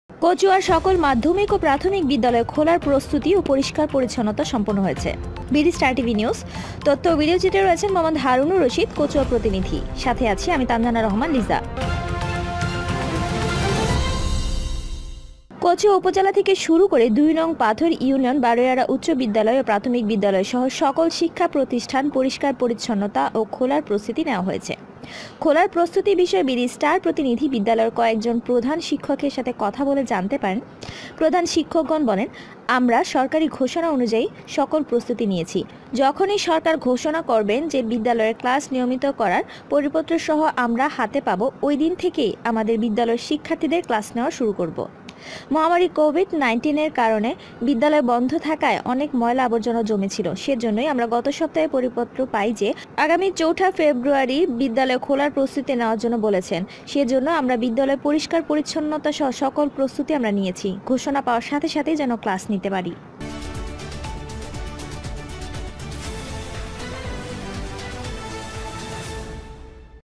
কচুয়ার সকল মাধ্যমিক ও প্রাথমিক বিদ্যালয় খোলার প্রস্তুতি ও পরিস্কার পরিচ্ছন্নতা সম্পূর্ণ
কচুয়ার সকল মাধ্যমিক ও প্রাথমিক বিদ্যালয় খোলার প্রস্তুতি ও পরিস্কার পরিচ্ছন্নতা সম্পূর্ণ হয়েছে